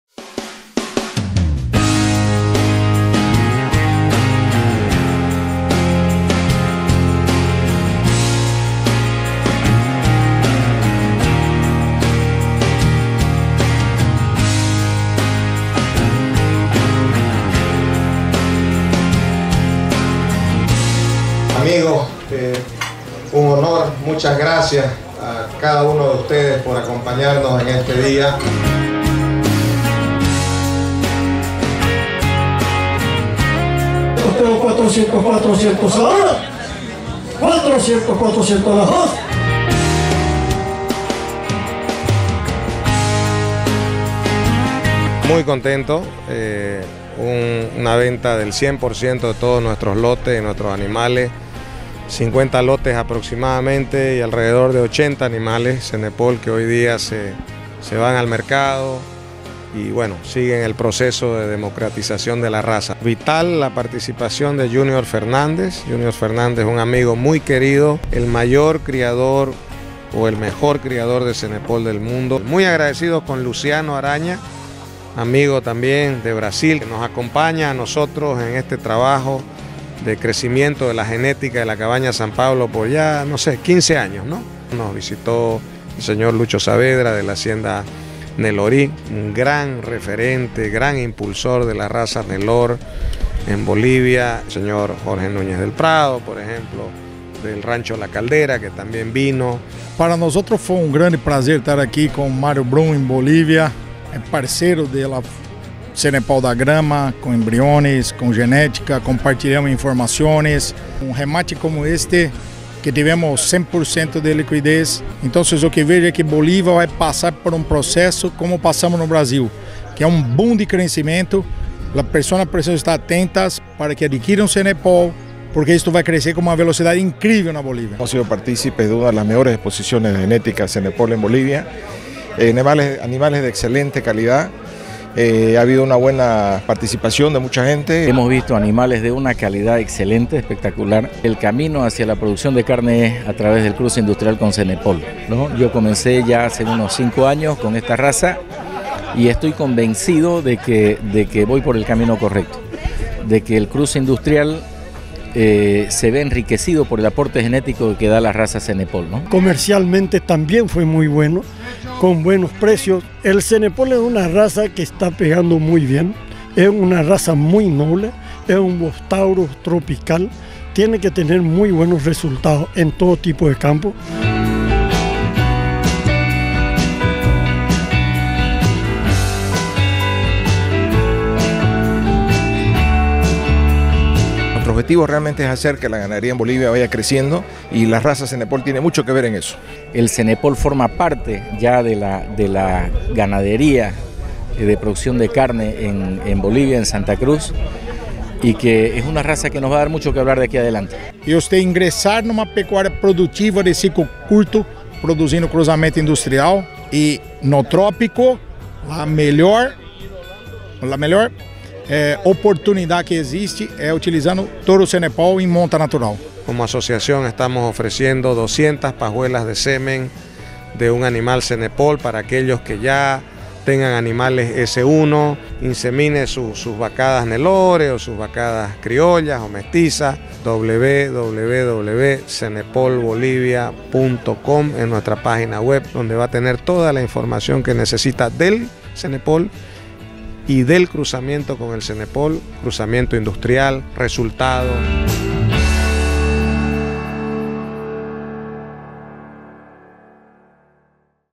CABAÑA SAN PABLO REALIZÓ EL REMATE SENEPOL MÁS GRANDE DEL PAÍS
La Cabaña San Pablo realizó en San Javier su Gran Remate de ganado Senepol. Evento que contó con un gran marco de espectadores de forma presencial y virtual. Conversamos con los protagonistas de esta actividad para conocer algunos detalles.